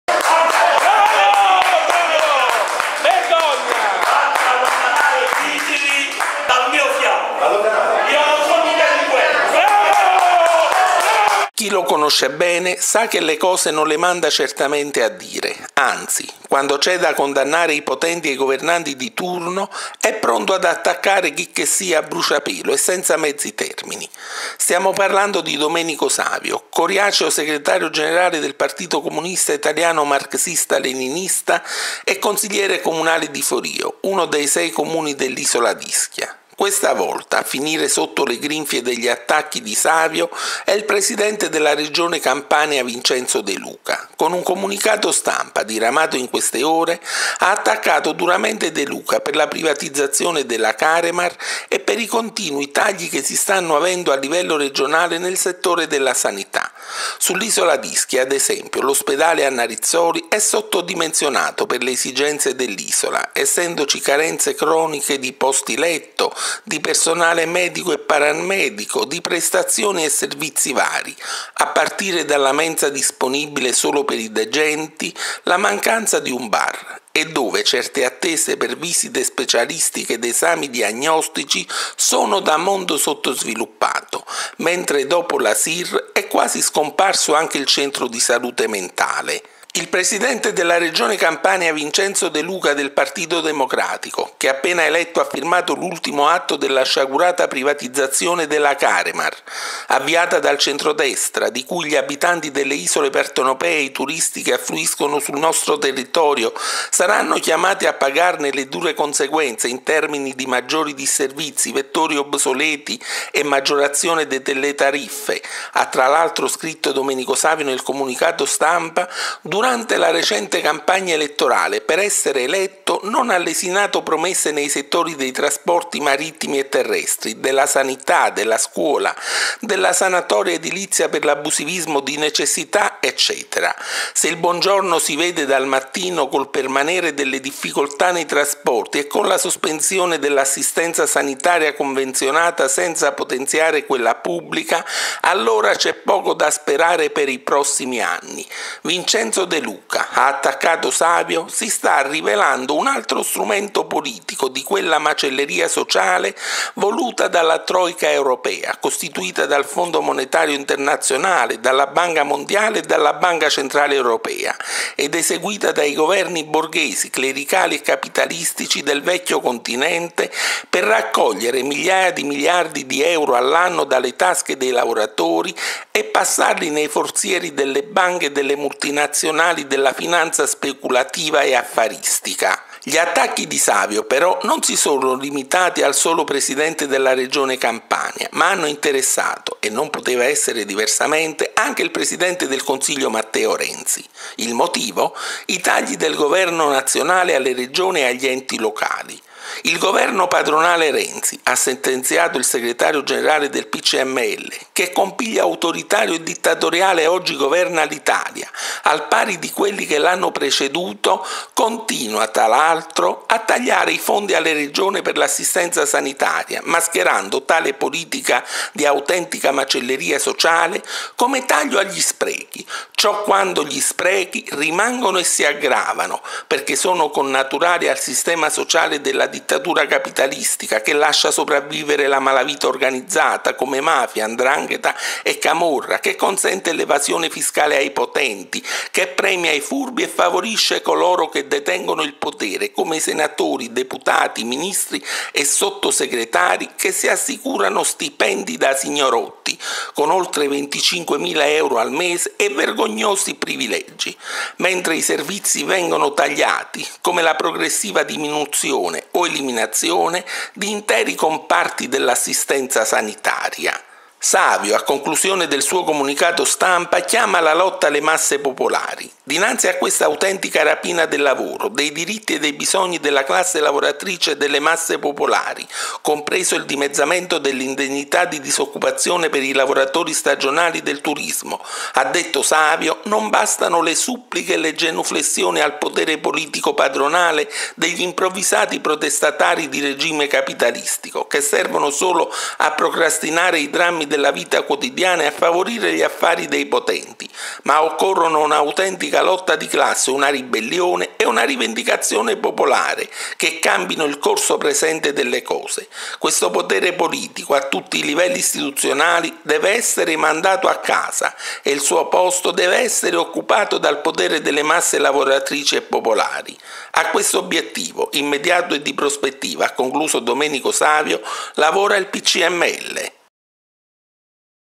PRIVATIZZAZIONE CAREMAR E TAGLI ALLA SANITA', DOMENICO SAVIO ATTACCA IL PRESIDENTE VINCENZO DE LUCA
PRIVATIZZAZIONE CAREMAR E TAGLI ALLA SANITA', DOMENICO SAVIO ATTACCA A BRUCIAPELO IL PRESIDENTE DELLA REGIONE:
“VINCENZO DE LUCA, UN ALTRO STRUMENTO DI 'MACELLERIA SOCIALE'!

Chi lo conosce bene sa che le cose non le manda certamente a dire, anzi. Quando c'è da condannare i potenti e i governanti di turno è pronto ad attaccare chicchessia a bruciapelo e senza mezzi termini. Stiamo parlando di Domenico Savio, coriaceo Segretario generale del Partito Comunista Italiano Marxista-Leninista e Consigliere comunale di Forio, uno dei sei comuni dell'isola d'Ischia. Questa volta a finire sotto le grinfie degli attacchi di Savio è il Presidente della regione Campania Vincenzo De Luca. Con un comunicato stampa diramato in queste ore, ha attaccato duramente De Luca per la privatizzazione della Caremar e per i continui tagli che si stanno avendo a livello regionale nel settore della sanità.  Sull'isola d'Ischia, ad esempio, l’ospedale “Anna Rizzoli” è sottodimensionato per le esigenze dell’isola essendoci carenze croniche di posti letto, di personale medico e paramedico, di prestazioni e servizi vari, a partire dalla mensa disponibile solo per i degenti, la mancanza di un bar e dove certe attese per visite specialistiche ed esami diagnostici sono da mondo sottosviluppato -, mentre dopo la SIR è quasi scomparso anche il Centro di Salute Mentale.
Ecco il comunicato stampa integrale di Domenico Savio:
      L’Italia è governata da forze politiche consimili di centrodestra e centrosinistra, che governano con tempestività e scrupolo gli affari del capitalismo e dell’imperialismo industriale, agrario, commerciale, bancario e finanziario nazionale, europeo e mondiale utilizzando pure lo spauracchio del debito pubblico per sottrarre in continuazione risorse economiche ingenti dalle tasche degli italiani, aggravandone progressivamente le già difficili condizioni di vita, per passarle nei forzieri della classe padronale. Per questa politica scellerata e antipopolare i governi di natura capitalistica continuano a tagliare i fondi – costituiti dalle tante tasse che i cittadini sono costretti a pagare - alle regioni, ai comuni e ai servizi pubblici, come scuola, sanità, trasporti, assistenza sociale e diritti dei lavoratori occupati e disoccupati. Contemporaneamente ai tagli prosegue il disegno politico perverso della privatizzazione di beni e servizi della collettività.